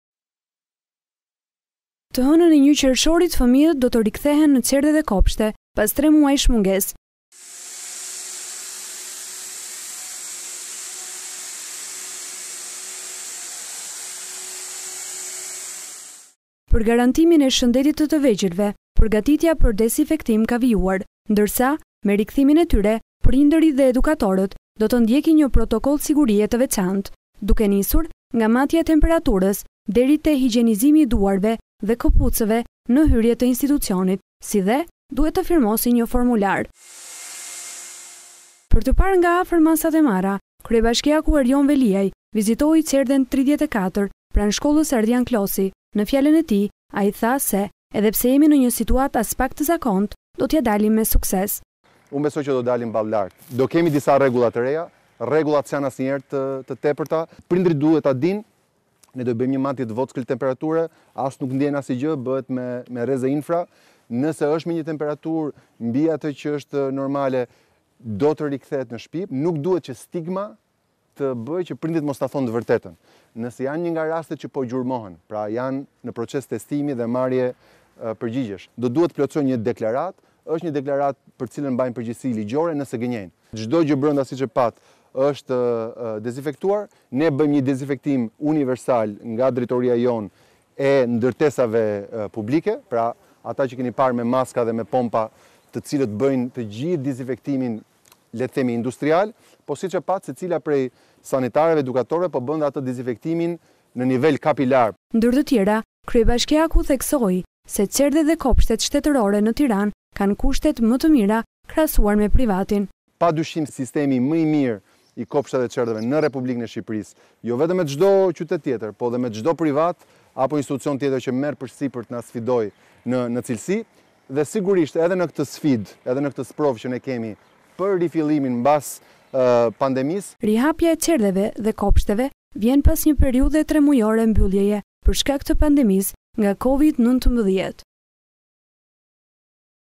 Report TV -Matje temperature, higjienizim të duarve dhe këpucëve në çerdhe dhe kopshte
Të hënën e 1 Qershorit fëmijët do të rikthehen në çerdhe dhe kopshte pas 3 muajsh të mbyllura. Përgatitja ka vijuar në mënyrë që të hënën këto institucione të jenë gati për të pritur fëmijët e vegjël. Matja e temperaturës, higjienizimi i duarve dhe këpucëve janë që në hyrje të çdo çerdhe dhe kopshti. Gjithashtu rekomandohet që fëmijët t'i sjelli vetë një prind dhe jo si familje, për më tepër të shmangen gjyshërit që janë dhe shtresa më vunerabël kundjet virusit.

Kryebashkiaku Erion Veliaj ishte në çerdhet 34, pranë shkollës “Ardian Klosi”, për të inspektuar masat shtesë që garantojnë shëndetin e fëmijëve, që nga dezinfektimi i mjediseve dhe deri tek ushqimi apo matja e temperaturës. Ai theksoi se edhe nga kjo situatë aspak e zakontë, që ka prekur gjithë botën, do të dilet ballëlartë, ndërsa falenderoi personelin për përkushtimin e treguar gjatë kësaj periudhe.